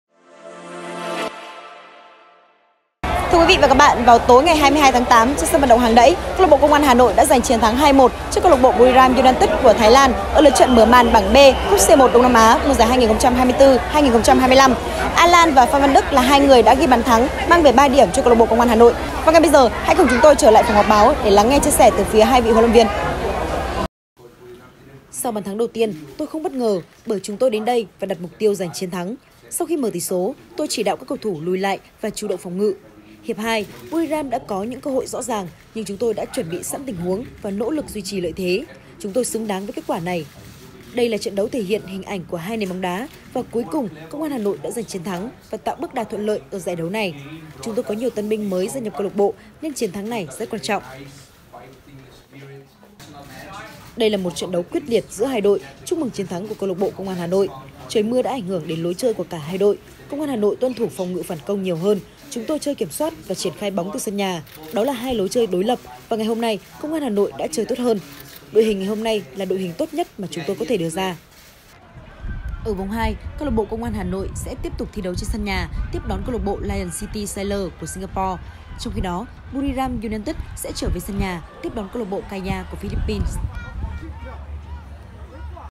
HLV Polking: Công an Hà Nội thắng Buriram United xứng đáng | Báo Lao Động
BÁO LAO ĐỘNG | Huấn luyện viên Mano Polking khẳng định, câu lạc bộ Công an Hà Nội xứng đáng với chiến thắng 2-1 trước Buriram United.

* Đường dây nóng: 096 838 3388